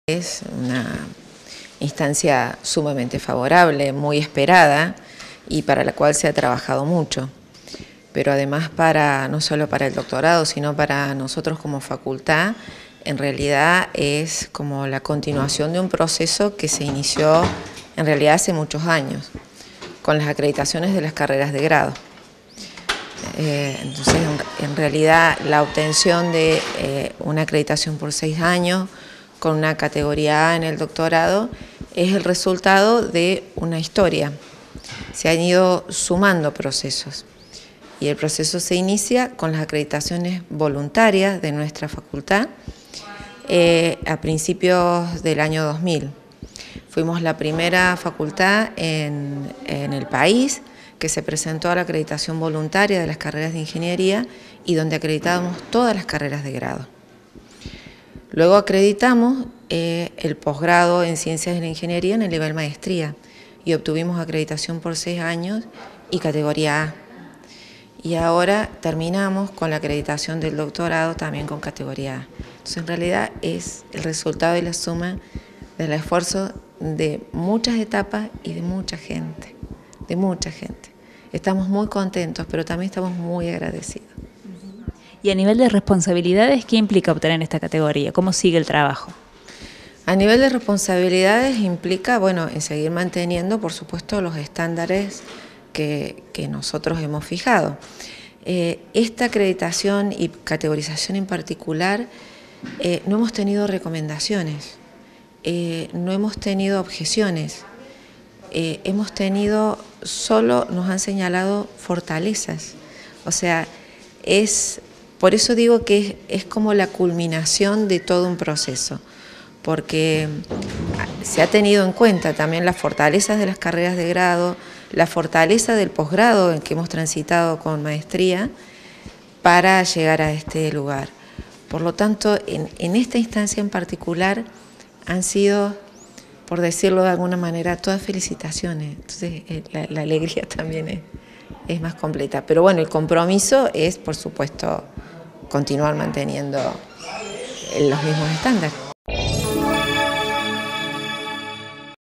Universidad Informa - Acreditación de carreras - Mercedes Carnero
MERCEDES CARNERO – SECRETARIA POSGRADO. EL DOCTORADO EN CIENCIAS DE LA INGENIERÍA OBTUVO CATEGORÍA “A”.

Facebook.com/uniriotv
Twitter.com/uniriotv
Instagram.com/uniriotv
Web - uniriotv.unrc.edu.ar
o envianos un email a
UniRio TV
Universidad Nacional de Río Cuarto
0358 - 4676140